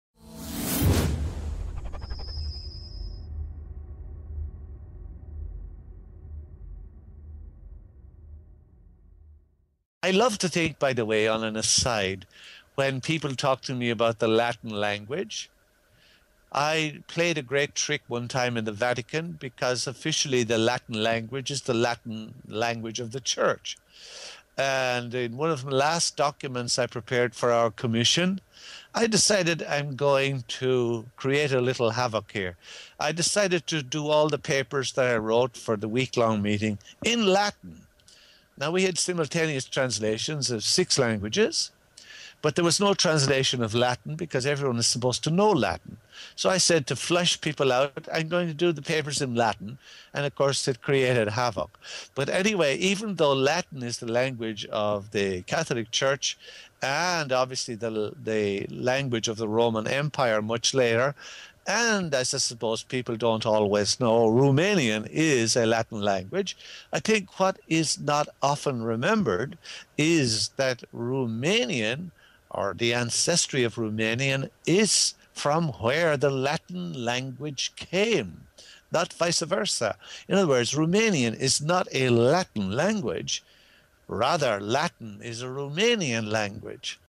About Romanian language
A short clip from an interview by Miceal Ledwith on a Romanian TV channel.
Miceal Ledwith was former adviser to Pope John Paul II, dean of St. Peter's College in Wexford Diocesan, former chairman of the Conference of Heads of Irish Universities and former member of the leadership of the European University Rectors Conference.